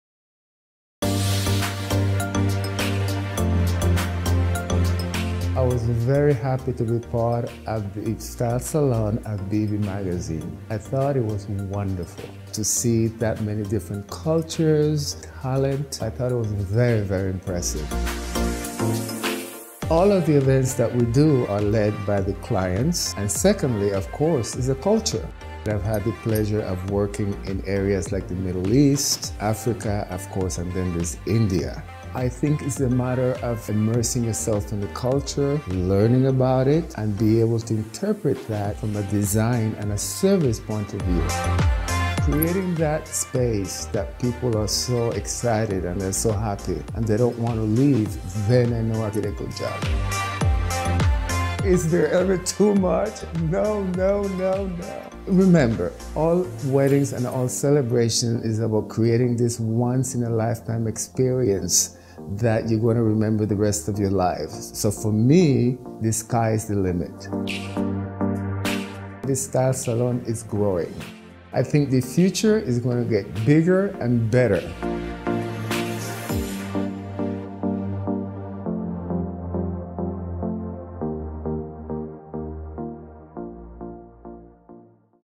The Style Salon with Preston Bailey 2019
The Style Salon is New York City's Multicultural Bridal Event that celebrates the diverse and vibrant bridal cultures of the city. The event showcases the multicultural bridal industry through presentations, design, fashion, catering and wedding entertainment. The 2019 Style Salon was headlined by Preston Bailey and was held at The Manhattan Manor in Times Square. Presented by Bibi Magazine. Video by Audry Black. TheStyleSalon.NYC